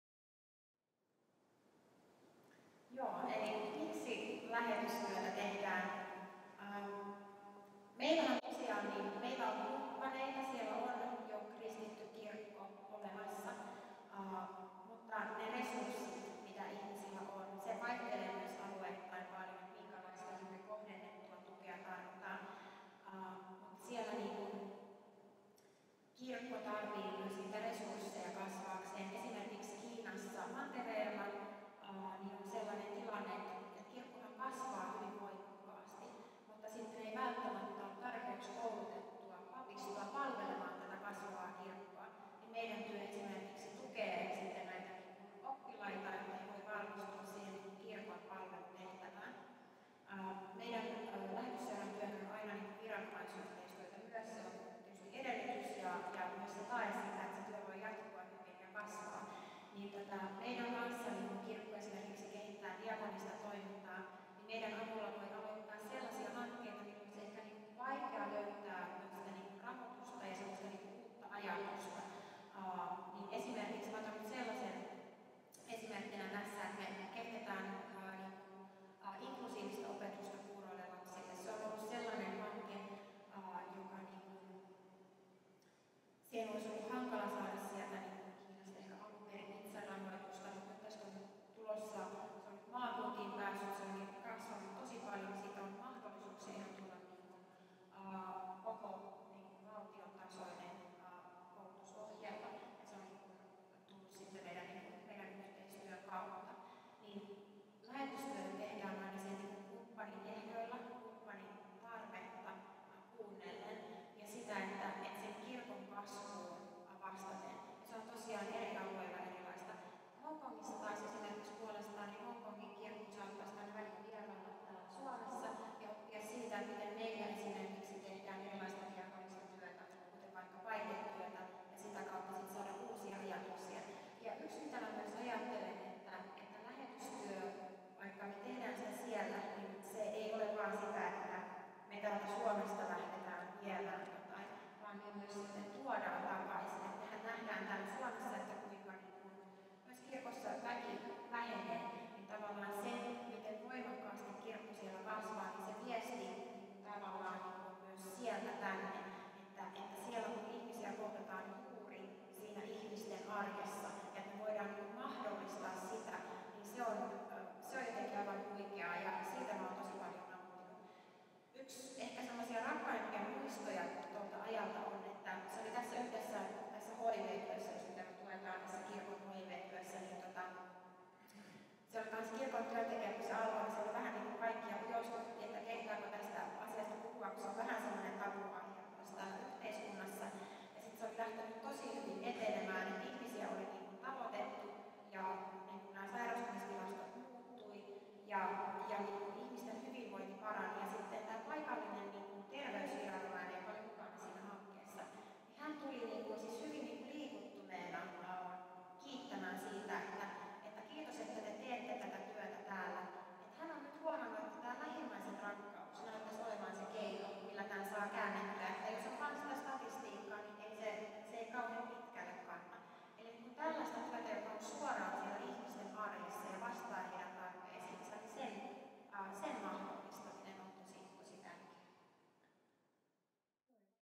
Miksi lähetystyötä tehdään? Lähetysseuran aluejohtaja Ilse Paakkinen
Suomen Lähetysseuran Honkongin ja Kiinan aluejohtaja Ilse Paakkinen vieraili Kallion messussa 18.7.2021. Kirkkokahvien haastattelussa hän kertoo, mitä merkitystä lähetystyöllä on ja miten se auttaa paikallisia kirkkoja.